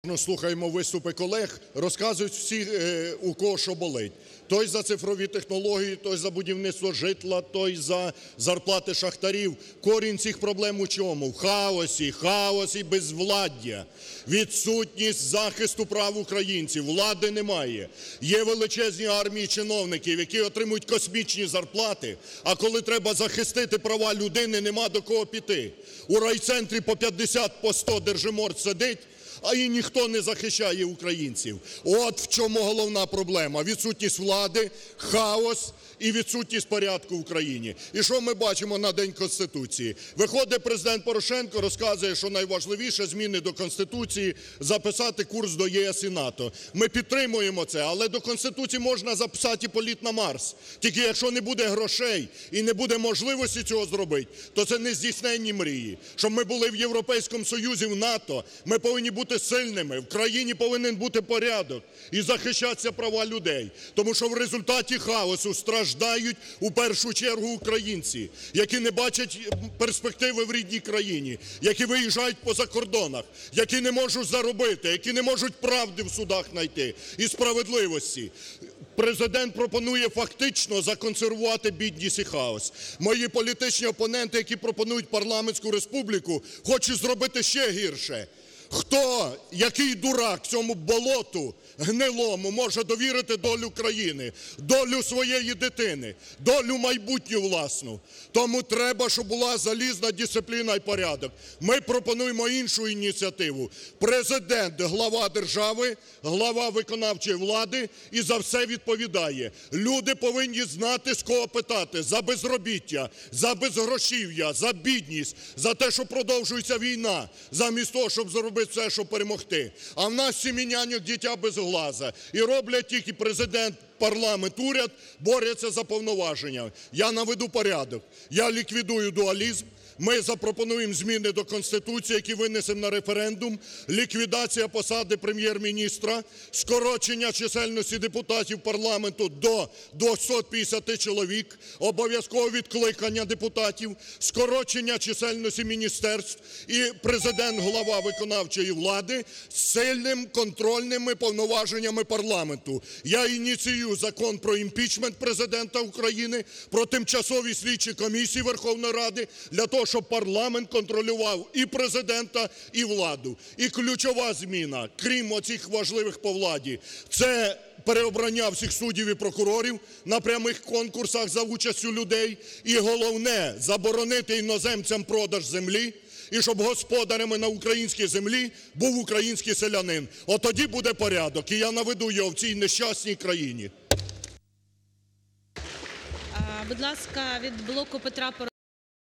Ляшко: Люди повинні знати, з кого питати за безгрошів'я і війну
Ліквідувати хаос і беззаконня в країні можна тільки тоді, коли буде один відповідальний – Президент, переконаний Олег Ляшко. Саме тому політик пропонує ухвалити зміни до Конституції і прибрати дуалізм у владі. Свої ініціативи лідер Радикальної партії озвучив з трибуни парламенту.
«На День Конституції виходить Президент Порошенко і розповідає, що найважливіше прописати в Основному Законі курс на ЄС і НАТО. Це, звичайно, важливо, але так можна в Конституції прописати і політ на Марс. І це залишиться нездійсненними мріями. Щоб стати членами ЄС і НАТО – треба бути сильними. В країні повинен бути порядок і захист прав людей. А у нас хаос, через що страждають українці, які не бачать перспектив в рідній країні і виїжджають на заробітки за кордон, не можуть знайти ні правди, ні справедливості у судах. І Порошенко пропонує фактично законсервувати бідність і хаос, а мої політичні опоненти, які пропонують парламентську республіку – хочуть зробити ще гірше. Який дурак цьому болоту гнилому може довірити долю країни та долю своїх дітей? Треба «залізною» рукою наводити порядок і дисципліну!», - наголосив Ляшко.
Лідер РПЛ знає, як покласти край хаосу в країні.
«Президент – голова держави і виконавчої влади. Він має за все відповідати. Люди повинні знати, з кого питати за безробіття, за безгрошів'я, за війну, яка затягується. А у нас у «семи няньок дитя без ока»!  Президент, парламент і уряд боряться між собою за повноваження. Я ліквідую цей дуалізм. Ми пропонуємо зміни до Конституції, які винесем на референдум. Це ліквідація посади прем'єр-міністра, скорочення чисельності депутатів парламенту до 250 чоловік, обов'язкове відкликання депутатів, скорочення чисельності міністерств і сильний парламентський контроль за Президентом. Я ініціюю ухвалення закону про імпічмент Президента України і ТСК, щоб парламент контролював і Президента, і владу. І ключова зміна – переобрання всіх суддів і прокурорів на прямих конкурсах за участі людей і заборона продажу землі іноземцям, щоб господарем на українській землі був тільки український селянин. Тоді буде порядок в країні!», - підсумував Ляшко.